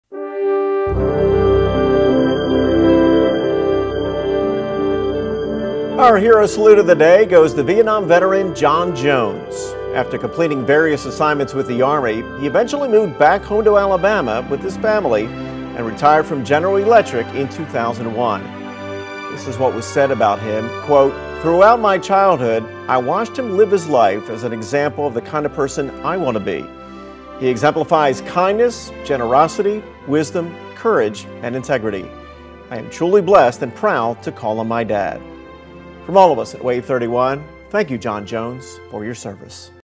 Hero Salute: John Jones
Vietnam Veteran John Jones is honored during the Hero Salute of the Day.